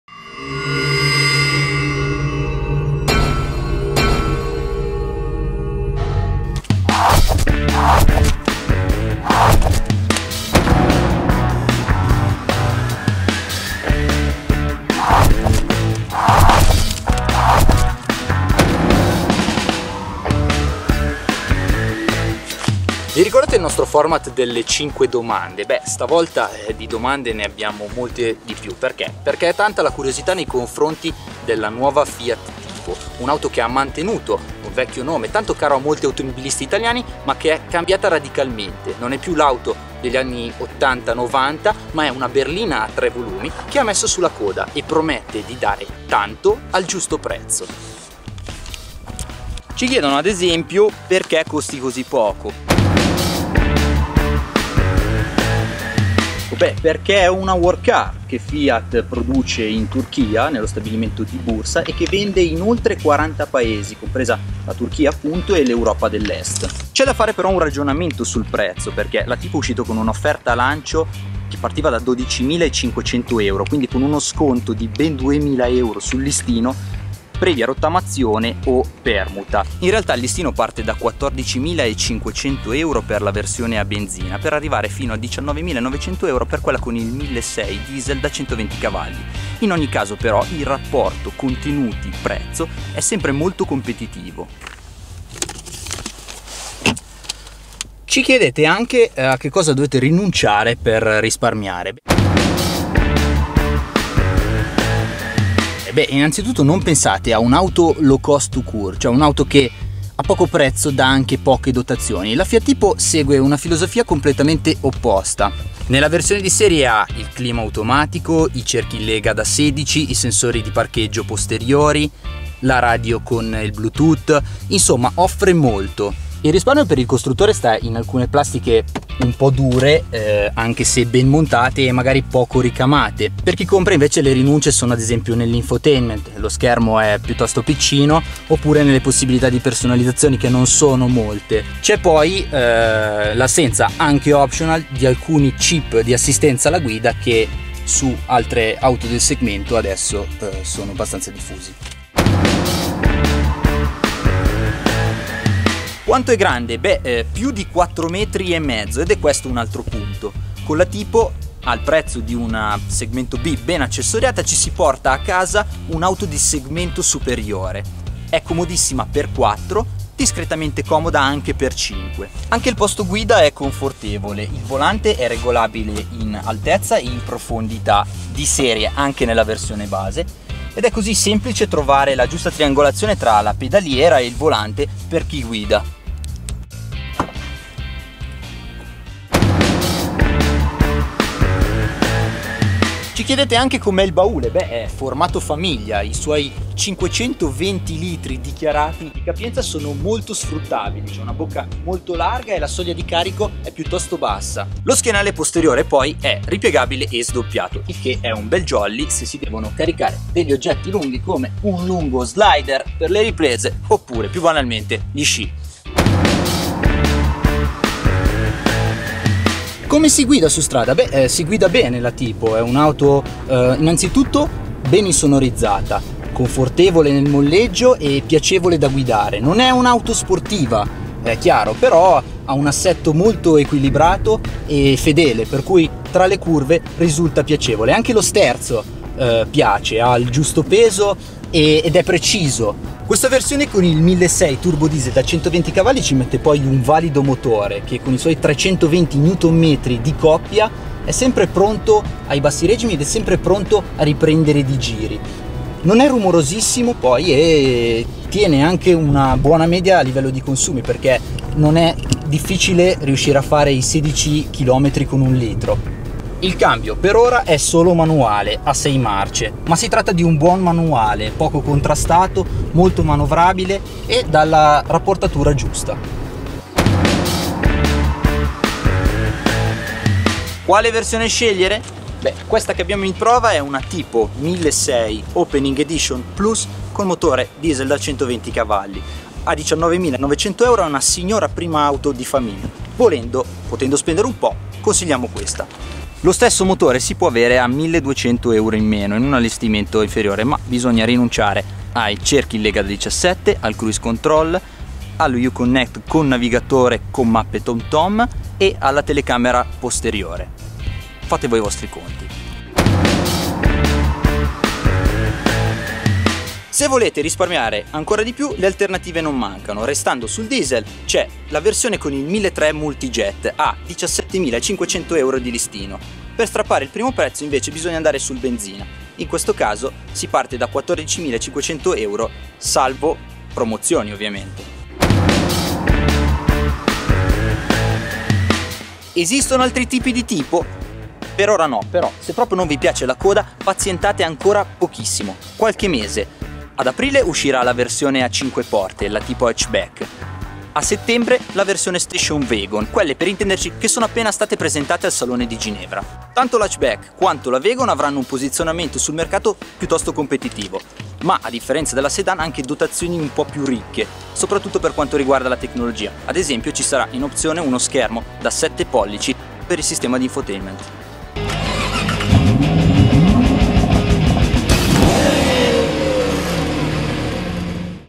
N Domande su... Fiat Tipo 4 porte
Agli italiani non piacciono le auto con la coda: sicuri? La nuova Fiat Tipo 4 porte ha un bel po' di argomenti per farvi cambiare idea...